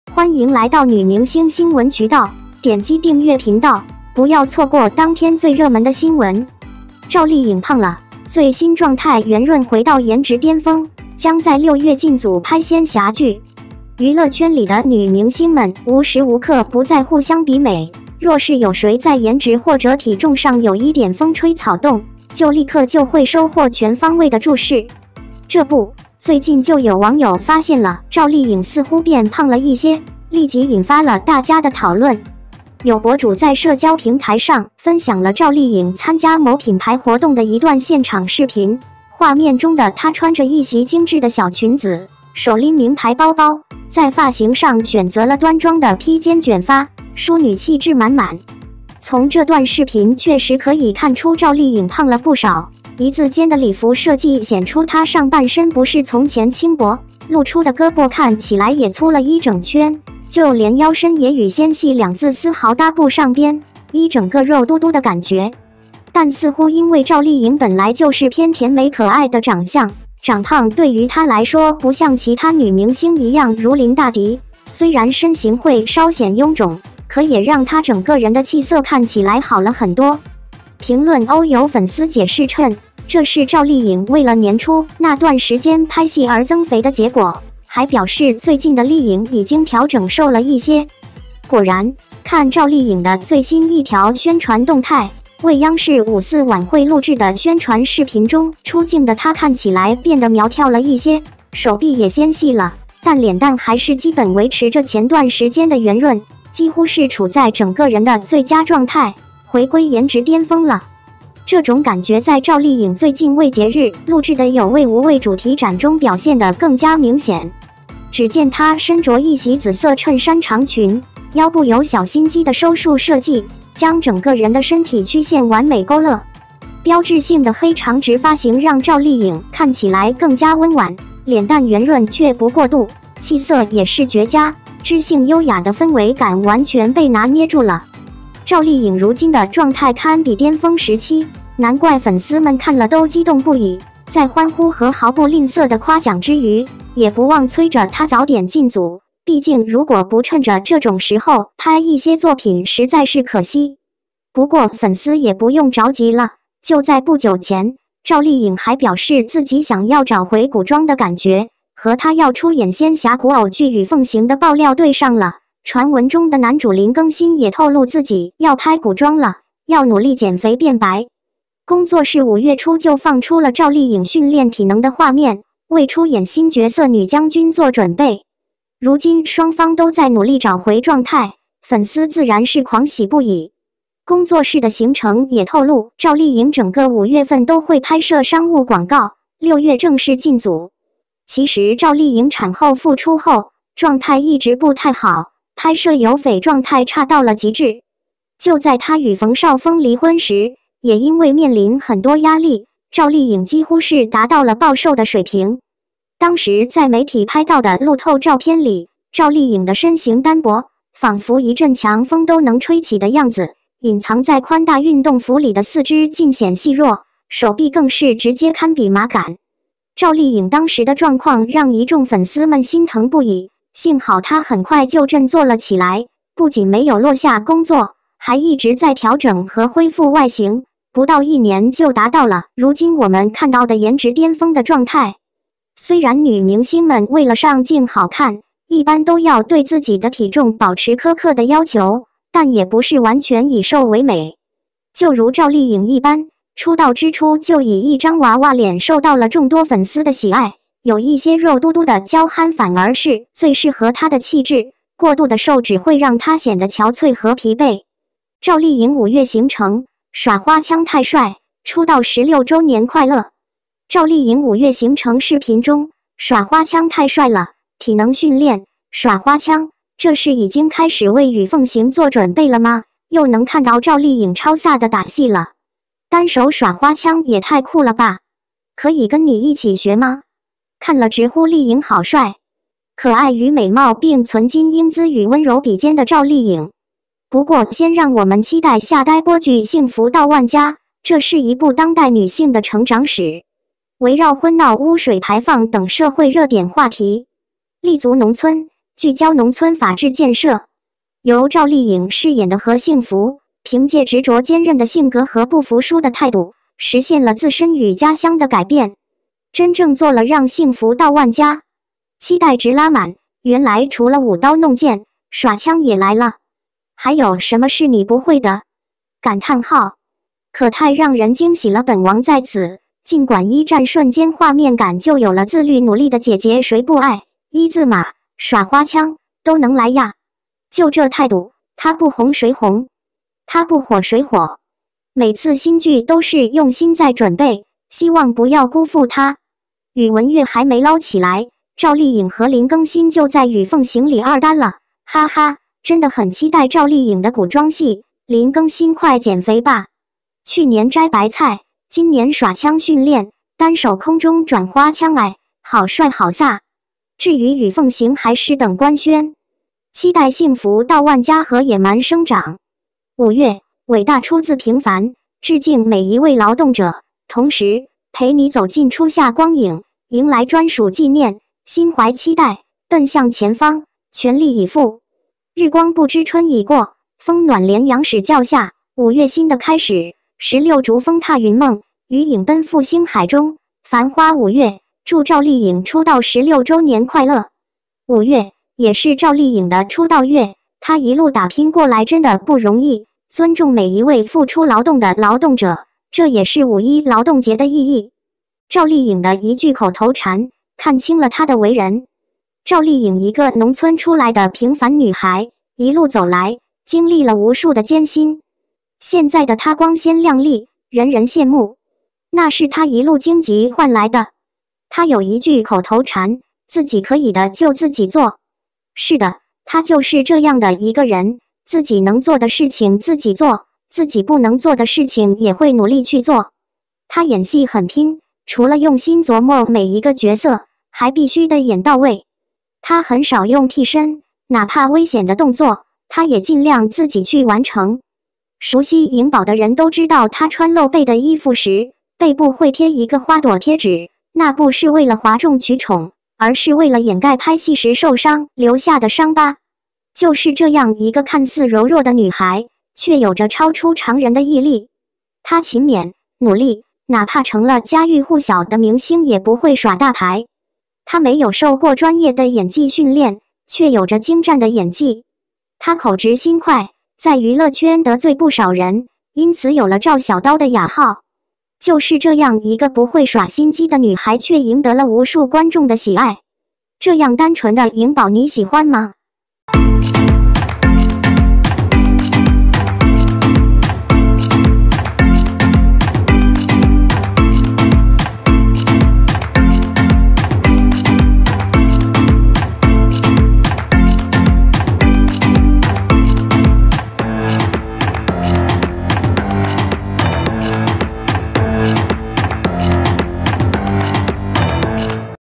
【女明星5月2日】赵丽颖胖了，将在6月进组拍仙侠剧, 赵丽颖五月行程，赵丽颖的一句口头禅，
赵丽颖胖了，最新状态圆润回到颜值巅峰，将在6月进组拍仙侠剧
赵丽颖五月行程，耍花枪太帅，出道十六周年快乐
赵丽颖的一句口头禅，看清了她的为人